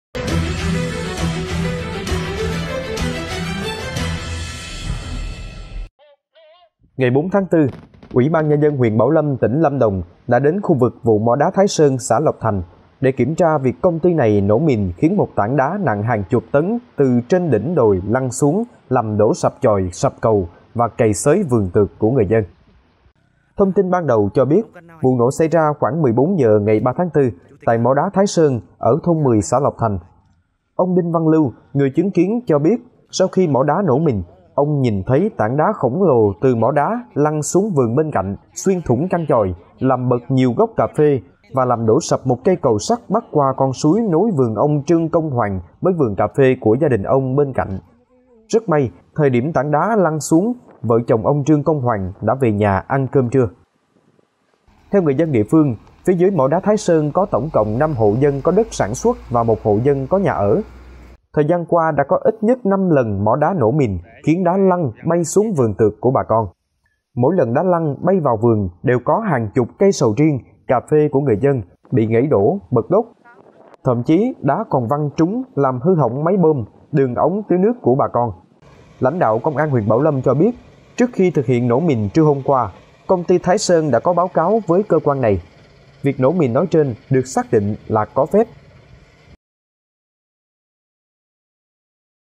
Bảo Lâm: Mỏ đá nổ mìn làm sập chòi, cày nát vườn tược của người dân
Hotline cung cấp thông tin: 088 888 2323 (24/24)